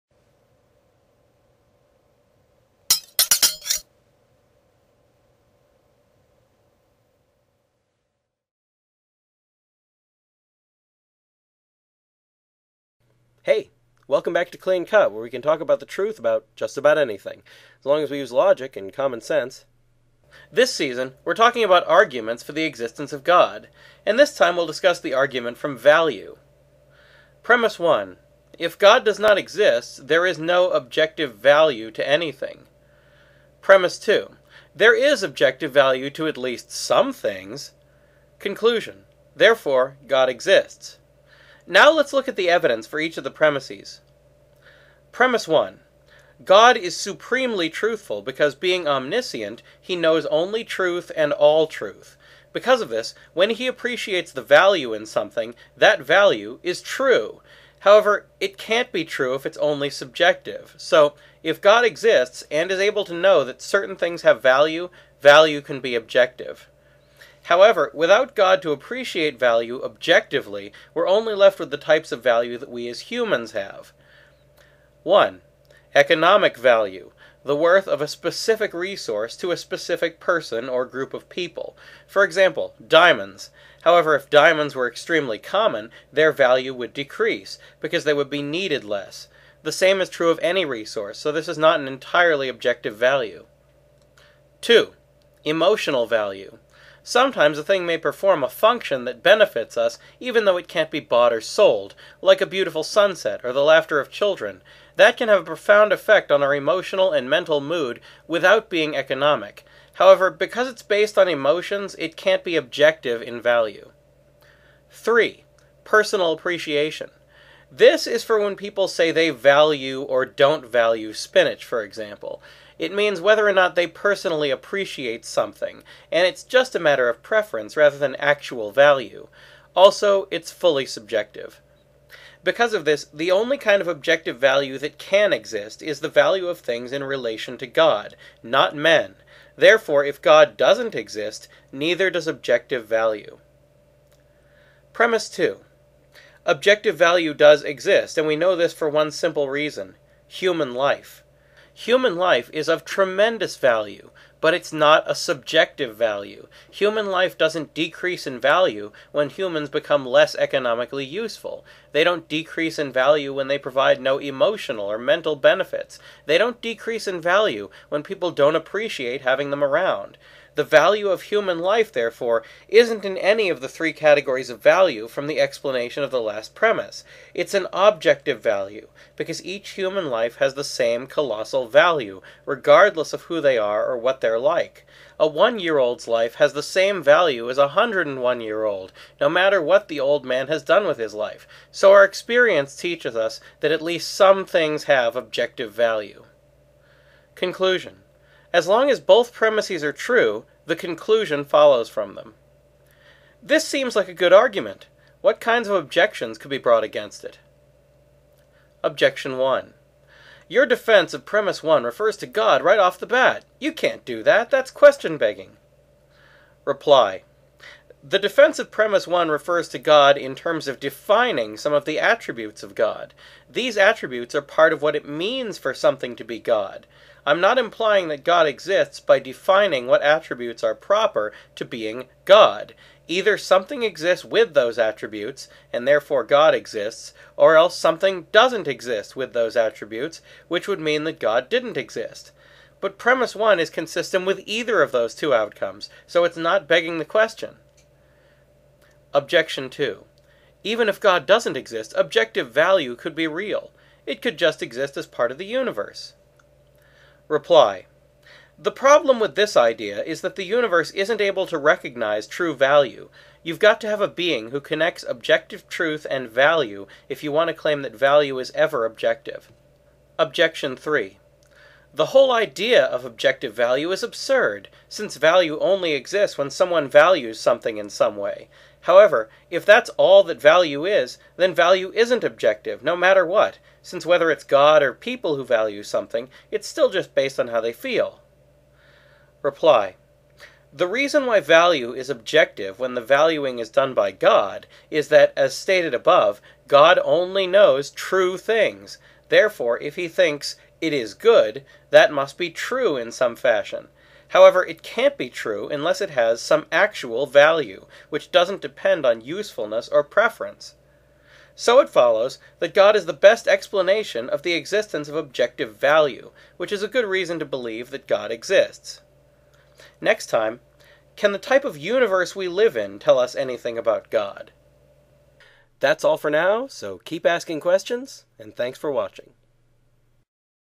Can Value Teach us About God?
Is there objective value?  What kinds of things have objective value?  Does objective value indicate anything about God?